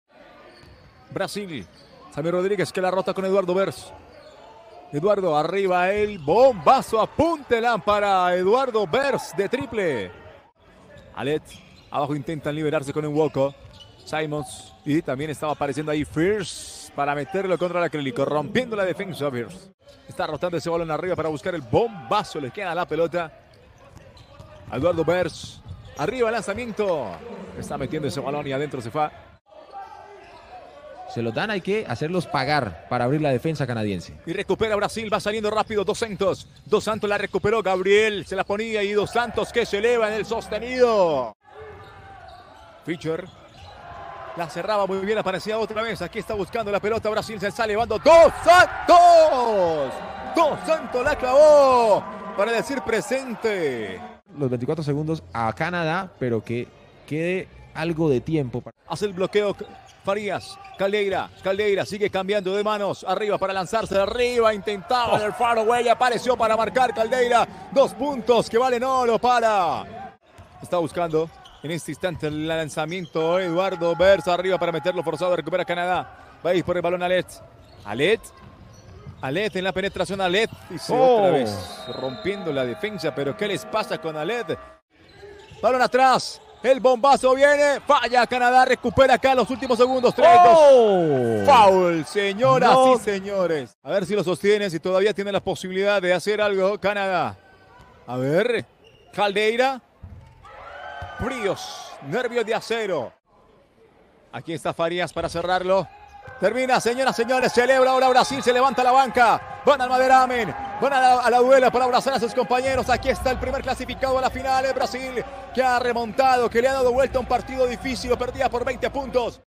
FIBA U18 Américas - Highlights - Brasil 78 x 77 Canadá
Veja a vitória do Brasil diante do Canadá pela AmeriCup 18 em Tijuana